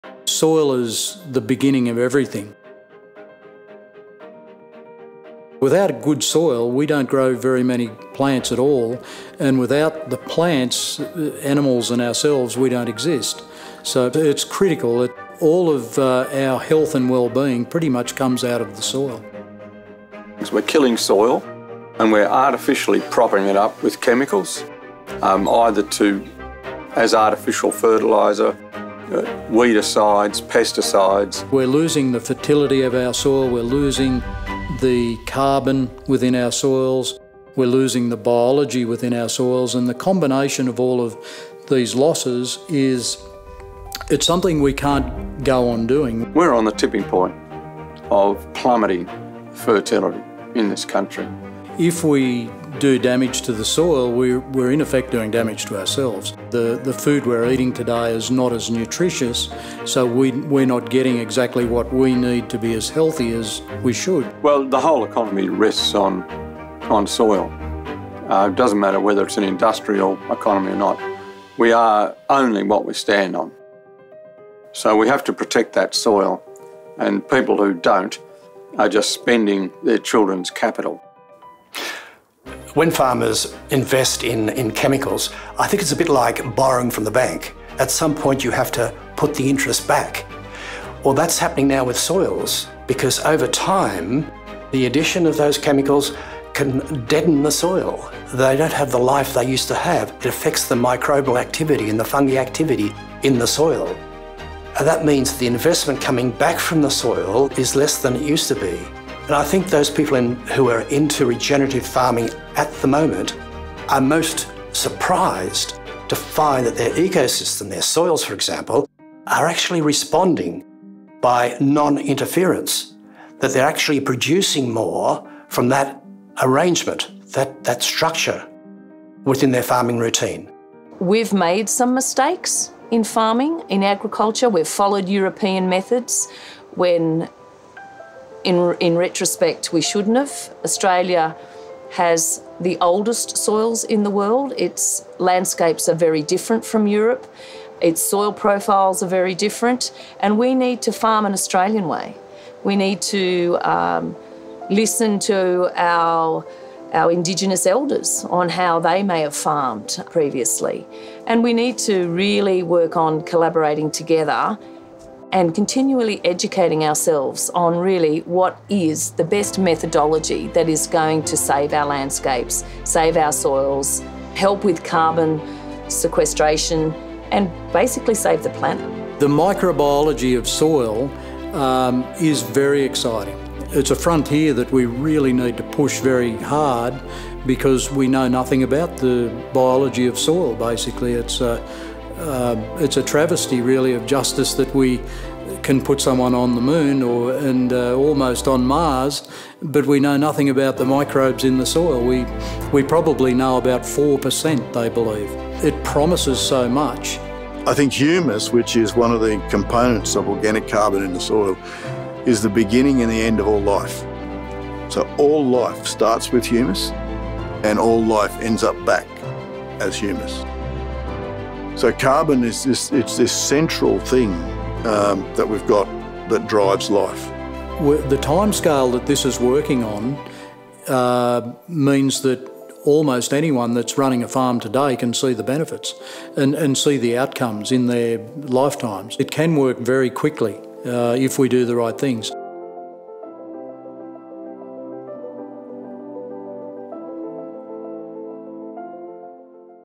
Importance of soil health in regenerative agriculture
Description:  Soil is the beginning of everything, without good soil we don’t grow healthy plants and we don’t eat good food, so it is critical we understand how to treat it more effectively. The whole economy rests on soils, so we have to protect that soil. The continued use of chemicals deadens the soil and affects the microbial and fungi activity in the soil making it less productive.   Farmers practising regenerative agriculture have noticed the positive results from non-interference in their farming routine. We need to improve our understanding of the microbiology of soil to make a difference.